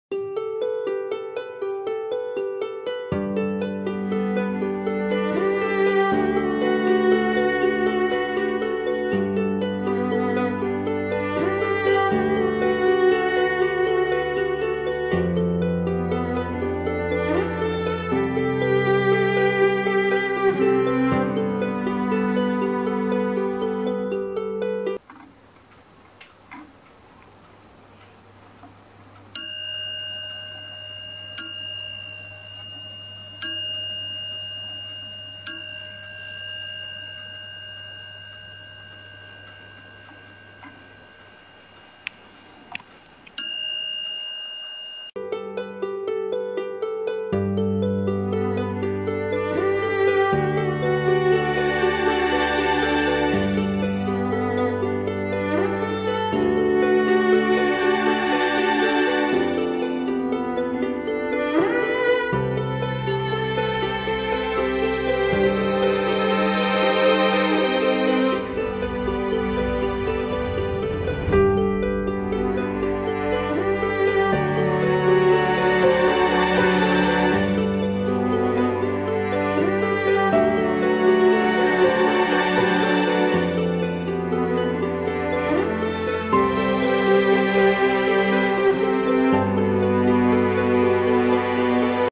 (No.740) นาฬิกาฮอลแลนด์ ยี่ห้อ WUBA ตุ้มแกว่งนอก ตีกระดิ่งบอกเวลาทุกๆครึ่งชั่วโมง | AM นาฬิกาโบราณ
เรื่องนาฬิกาโบราณ นาฬิกาไขลาน ต้องยกให้เรา
"AM นาฬิกาโบราณ"
add line "เพิ่มเพื่อน" มาติดตาม และพูดคุยกันได้ที่